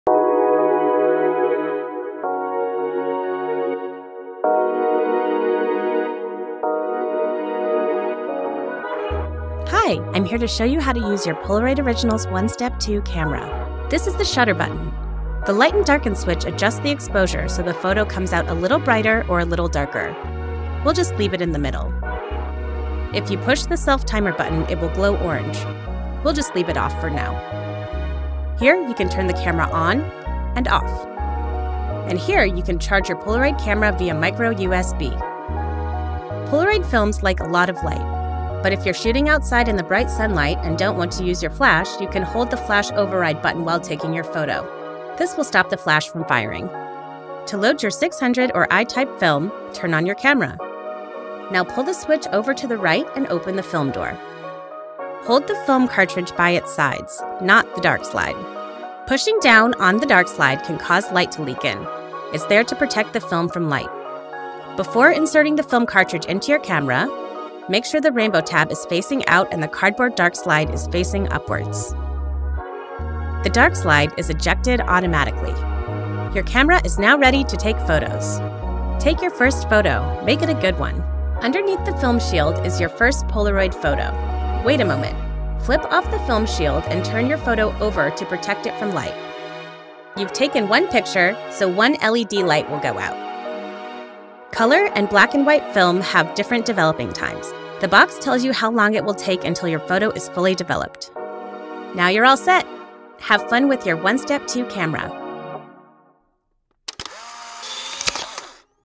How to take photos with the OneStep 2
In this video, we show you how to take photos with the Polaroid OneStep 2 camera.

So you’ve got your OneStep 2, and it’s loaded with film. So what next? Taking a picture, of course!

If you need a little help taking that first photo, this short video will walk you through the simple steps to using your OneStep 2. Then you’ll be ready to roll in no time. But remember – we can’t help you when it comes to what goes in the photo. You’re the artist, after all.